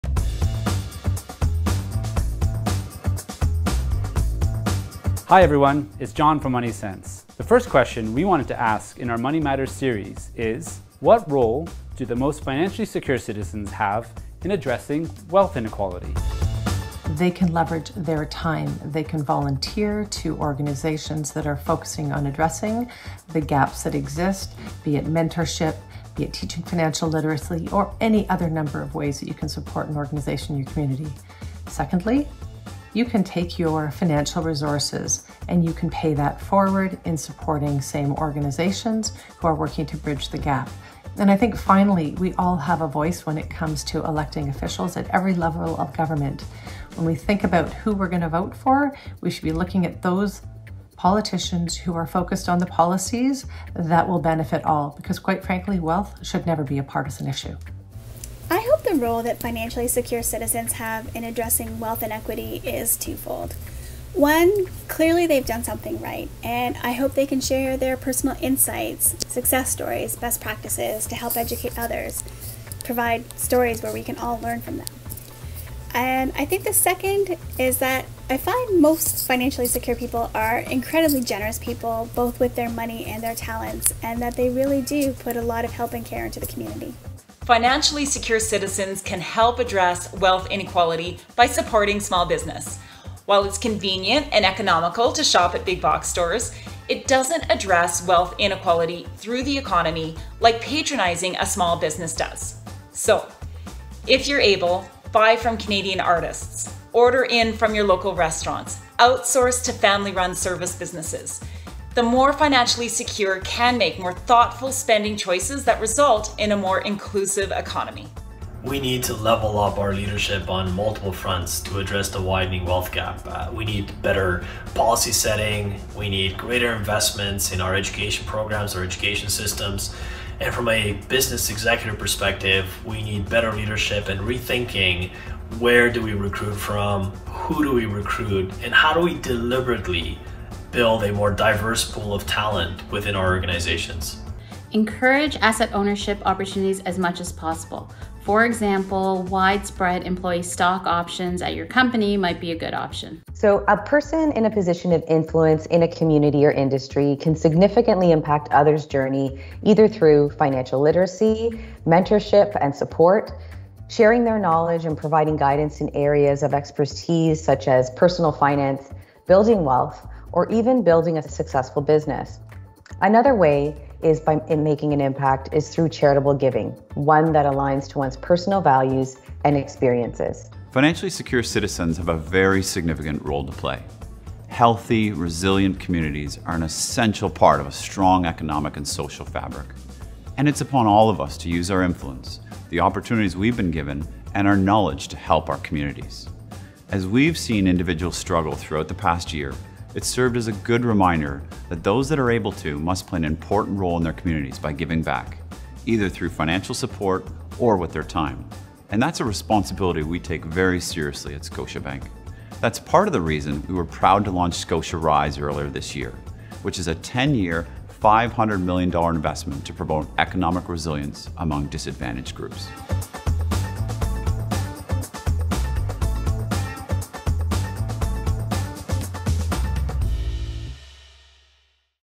What role do the most financially secure citizens have in addressing wealth inequality?
MoneySense partners share their answers to questions that we hope will inspire and inform Canadians about their personal finances.